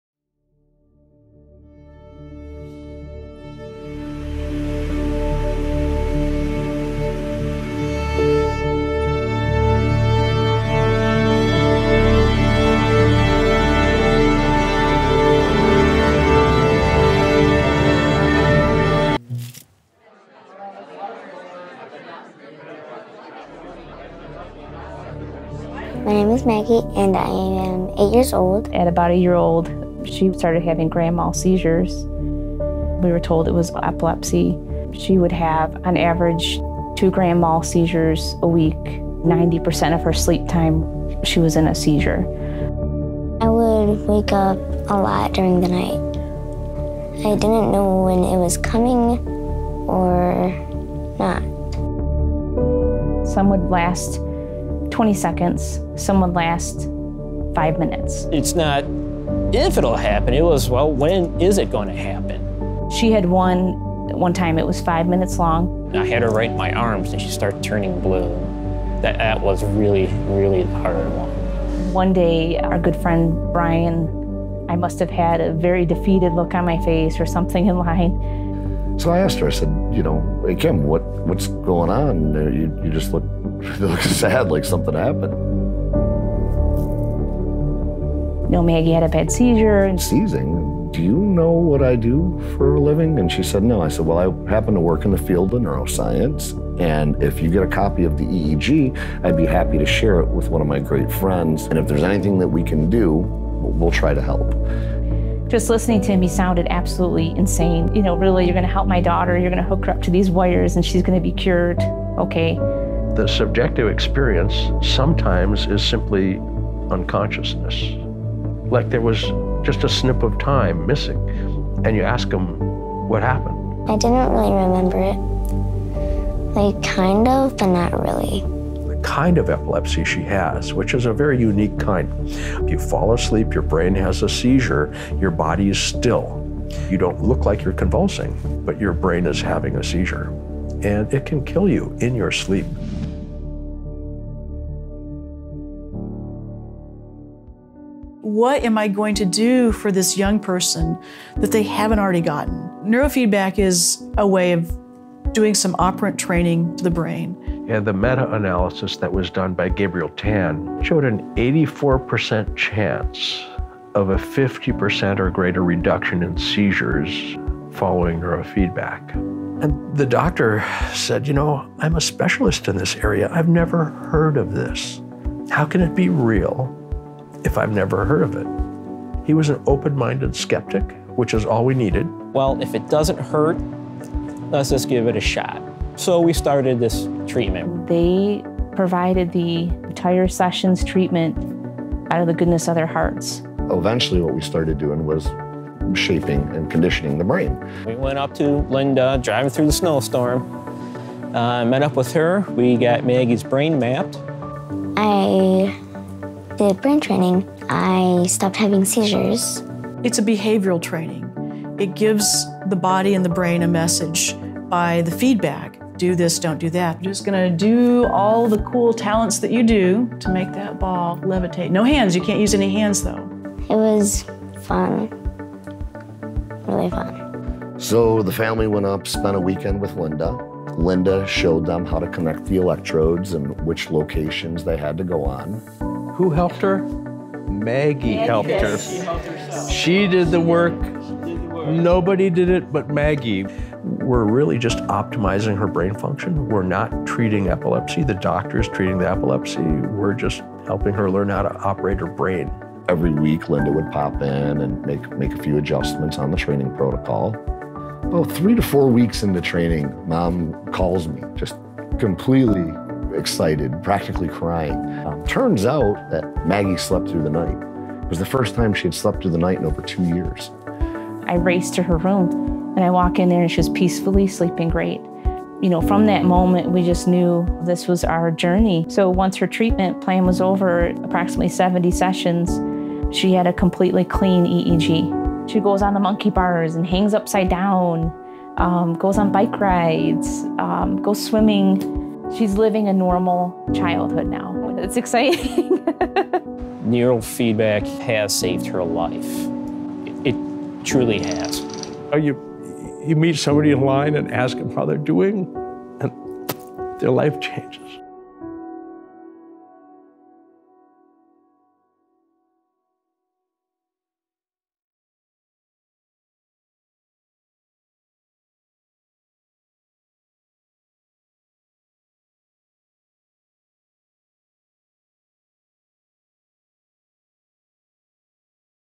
CASE STUDY NEUROFEEDBACK (napisy PL)
''Neurofeedback uratował jej życie.''

Maggie od najmłodszych lat miała napady padaczkowe, które występowały średnio dwa razy w tygodniu. W wieku 5 lat rozpoczęła treningi neurofeedback - obejrzyj materiał i przekonaj się sam jak neurofeedback może skutecznie pomagać i zmieniać ludzkie życie.